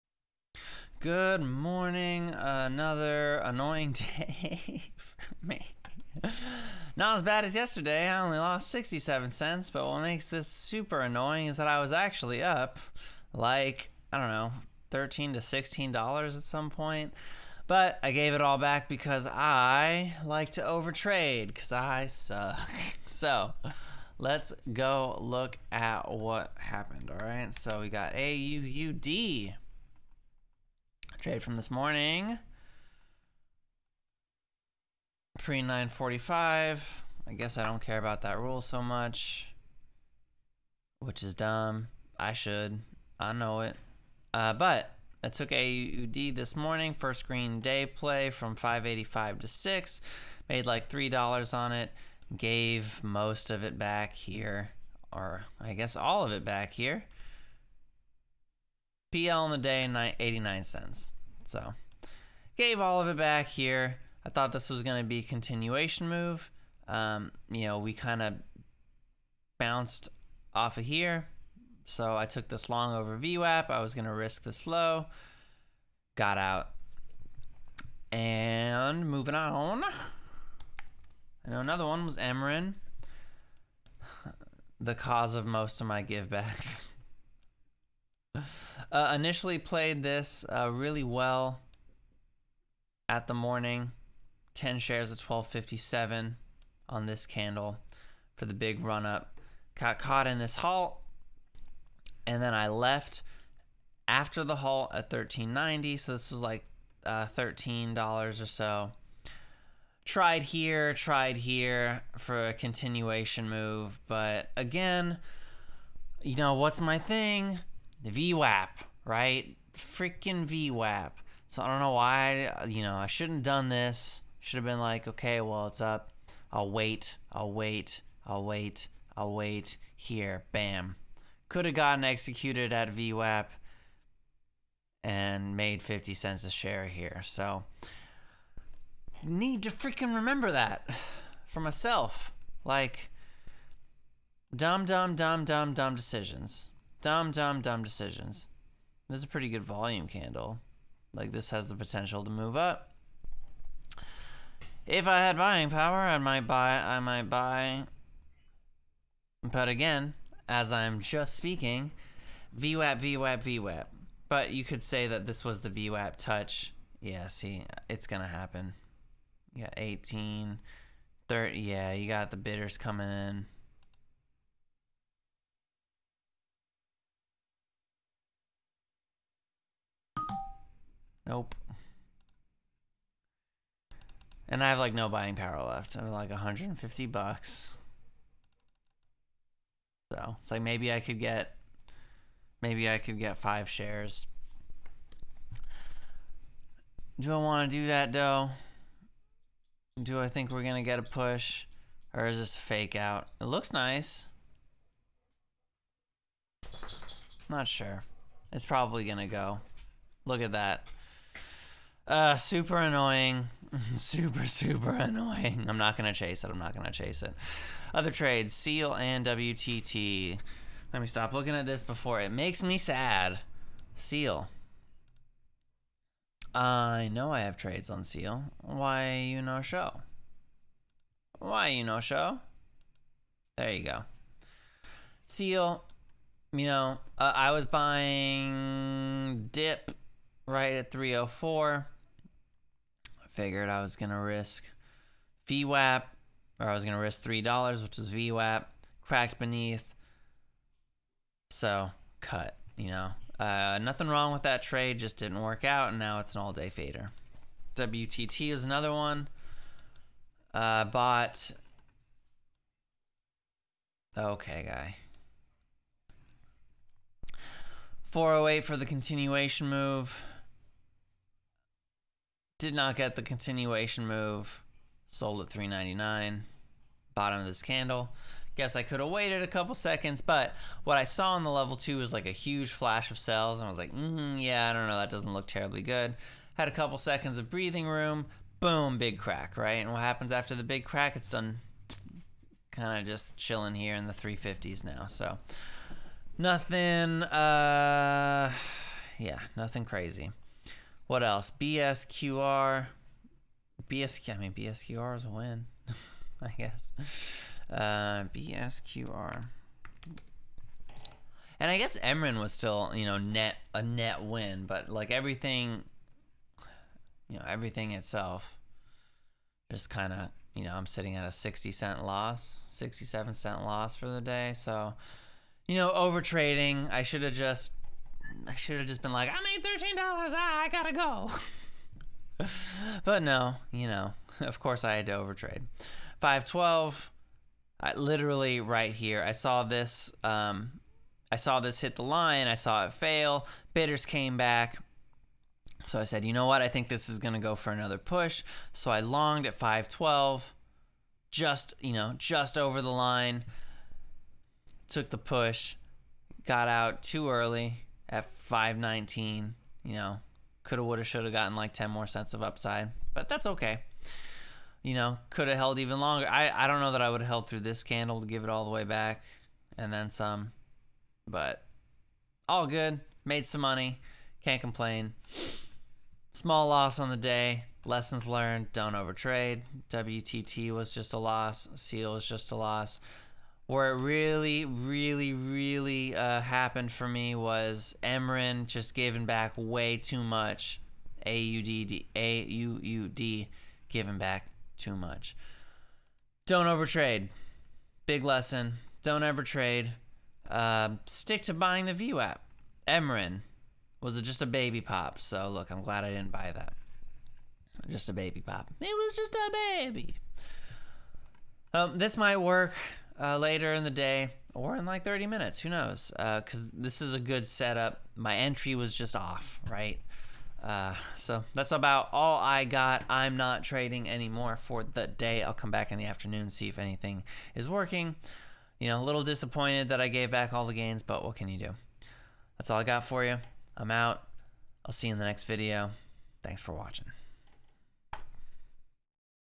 A lesson in overtrading
Great morning with good wins on $MRIN & $AUUD, but ended up with a small loss because of stupid overtrading & following entries without respect to my own rules. VWAP BOUNCE ONLY. HOW MANY TIMES DO I NEED TO LEARN THIS LESSON UGH.